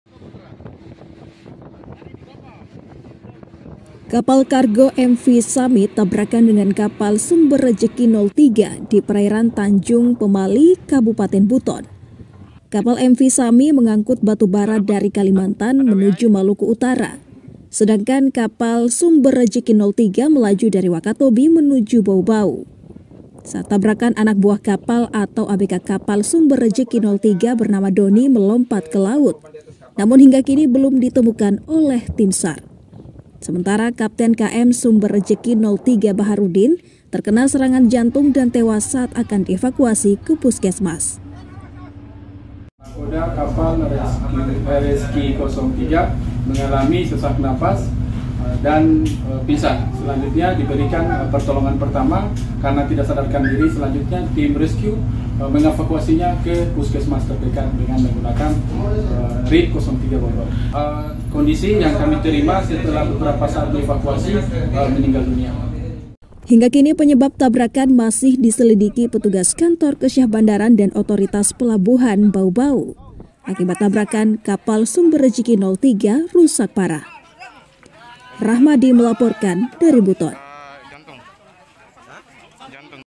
Tabrakan Kapal Di Tanjung Pemali Buton, Satu Tewas Dan Satu Abk Hilang Tenggelam||SCTV KENDARI
KAPAL KARGO MV SAMI TABRAKAN DENGAN KAPAL SUMBER REJEKI 03 DI PERAIRAN TANJUNG PEMALI KABUPATEN BUTON//
KAPAL MV SAMI MENGANGKUT BATUBARA DARI KALIMANTAN MENUJU MALUKU UTARA SEDANGKAN KAPAL SUMBER REJEKI 03 MELAJU DARI WAKATOBI MENUJU BAUBAU//
SAAT TABRAKAN / ANAK BUAH KAPAL ATAU ABK KAPAL SUMBER REJEKI 03 BERNAMA DONI/ MELOMPAT KE LAUT // NAMUN  HINGGA KINI BELUM DITEMUKAN OLEH TIM SAR //
SEMENTARA KAPTEN KM SUMBER REJEKI 03 BAHARUDDIN/ TERKENA SERANGAN JANTUNG DAN TEWAS SAAT AKAN DIEVAKUASI KE PUSKESMAS//
HINGGA KINI / PENYEBAB TABRAKAN / MASIH DISELIDIKI PETUGAS KANTOR KESYAHBANDARAN DAN OTORITAS PELABUHAN (KSOP) BAUBAU //
AKIBAT TABRAKAN / KAPAL SUMBER REJEKI 03 RUSAK PARAH//